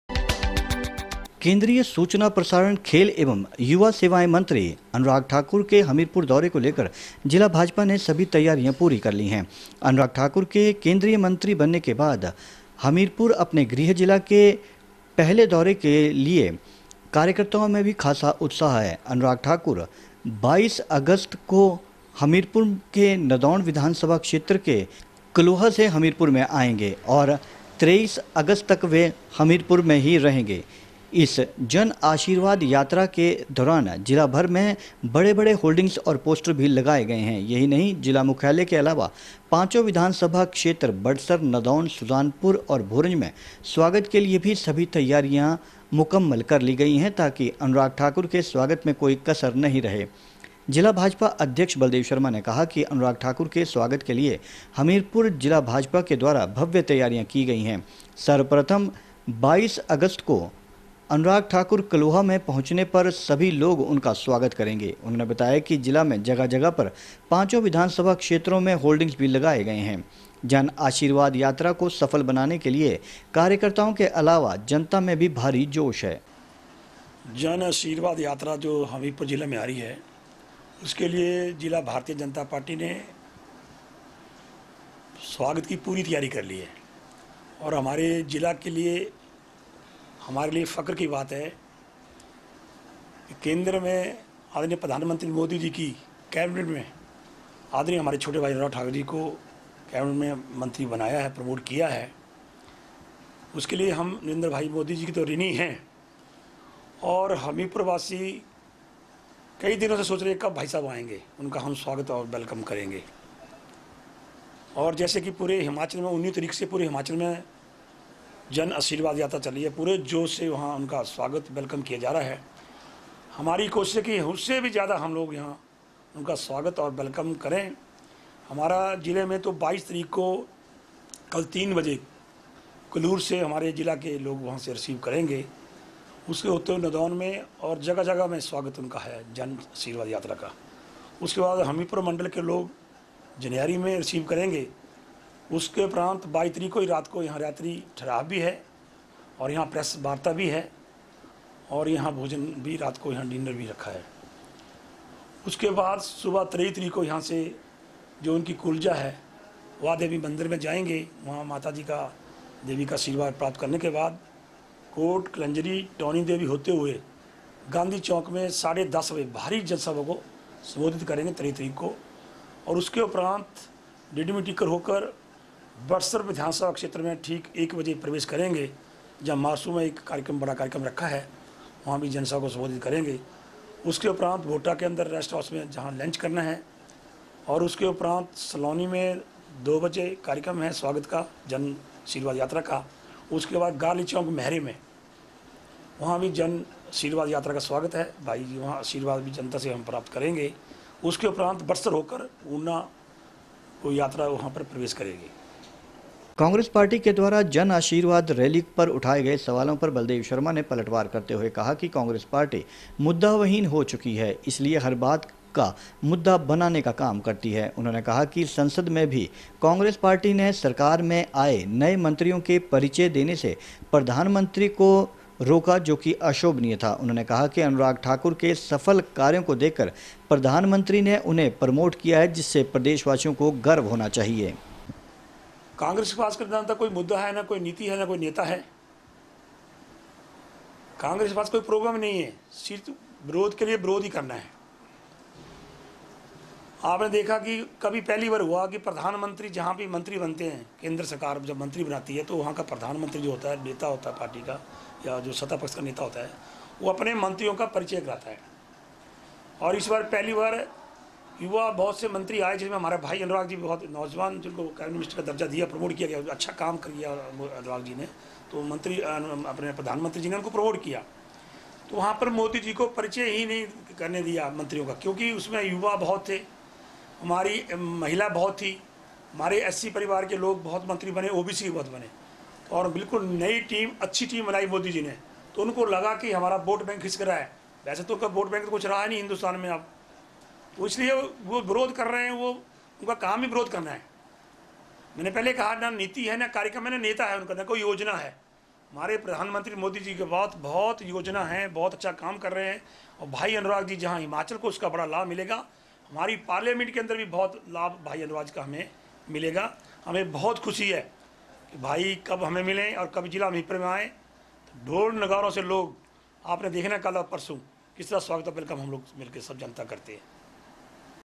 hamirpur BJP Anurag welcome
केंद्रीय मंत्री अनुराग सिंह ठाकुर की जन आशीर्वाद यात्रा के लिए हमीरपुर भाजपा ने तैयारियां की मुक्कमल, नादौन के कलूर में होगा अनुराग सिंह ठाकुर का भव्य स्वागत, जिलाध्यक्ष बलदेब शर्मा ने दी प्रेसवार्ता में दी जानकारी, बलदेब शर्मा ने कांग्रेस पर साधा निशाना, कहा कांग्रेस के पास न नेता न नीति।